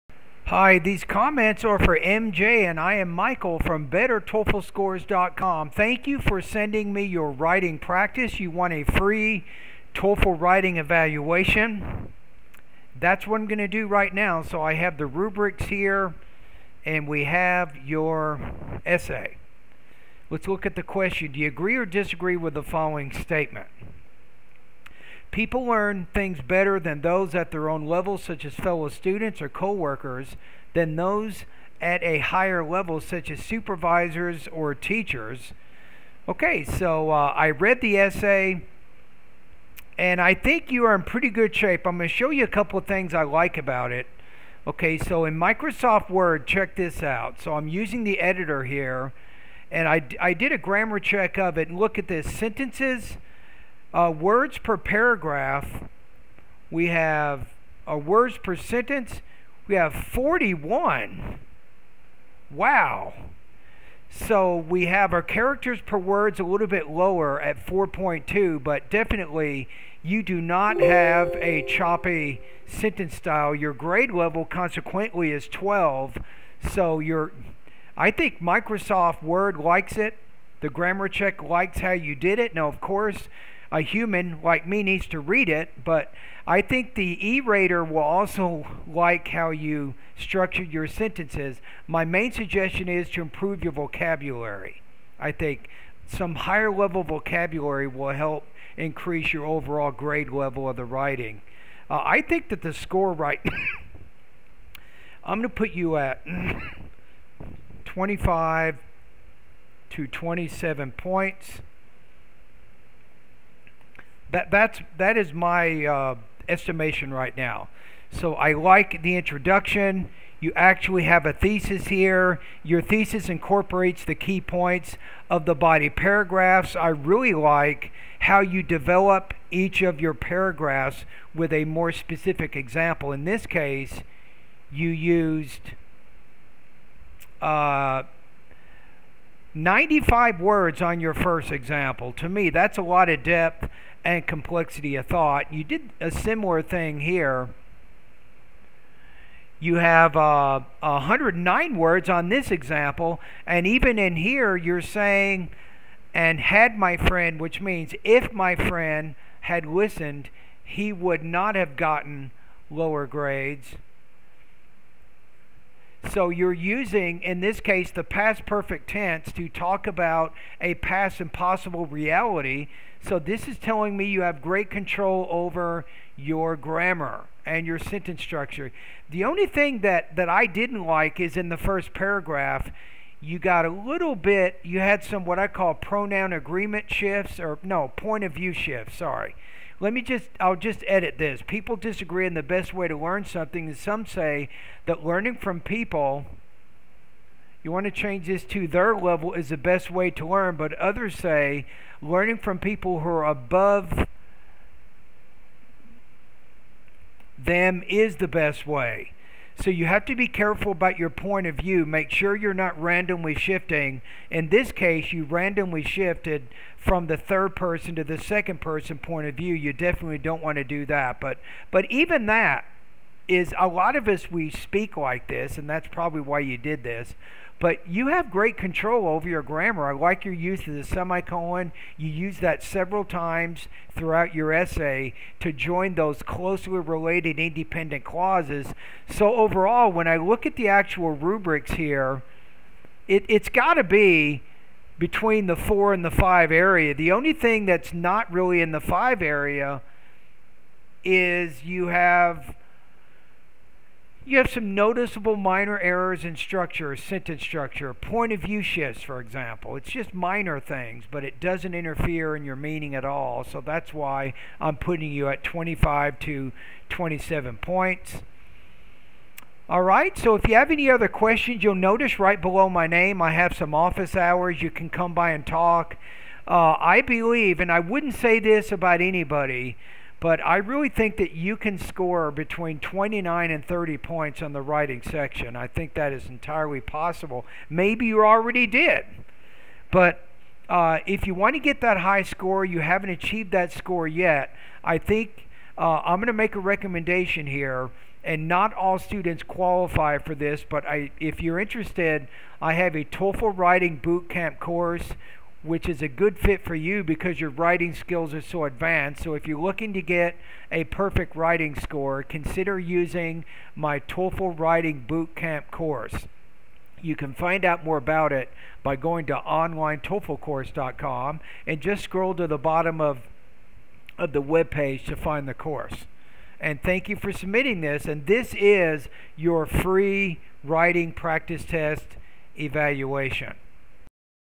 Free TOEFL writing fdbk for MJ: Your average sentence length is 41 words! Your score is 25-27 pts.
Your score 25-27/30 pts.

Learning is a process that all humans experienced, and we cannot do anything in this life without learning how to do it first. People disagree on the best way to learn something, and some say that learning from people at their level is the best way to learn, but others say learning from people who are levels above them is the best way. If I were asked, I Would say that learning from someone more experienced than you is better because they can teach you what others can't, and, also, some people do not like to be taught by normal people.

First of all, there is no doubt that experts know how to give you the information that you need. For instance, when I was in high school, my friend and I were preparing for the Math exam that we were going to take, and I invited my friend to my house in order to study together; nevertheless, we struggled to teach each other the difficult concepts, and we could not convey the information clearly to each other. The next day, I asked our teacher to teach us after the class, and when he did, I felt like those hard numbers were as easy as drinking water, and after the exam, we got perfect grades; therefore, it is obvious that experts know so much about their field that they can make the difficult things way easier for others.

Second, some people think that they will not learn anything new from people at their level, so they choose not to listen to them. For example, in high school, our English teacher used to make us work as groups, and he used to choose a student in each group to teach the lesson to their groups, but my friend thought that he would not learn anything from these students, so he used to ignore them. As a result, he did not understand any of the lessons, and he ended up recieving low grades on his English tests; therefore, no one can deny that because some people do not listen to people who are at their level, they will not learn anything, and had my friend listened, he would not have gotten low grades.

Thus, as it has been discussed, I fail to agree that people learn better from people at their level because experts know how to deliver information clearly, and, also, some people refuse to be taught by people with no experience.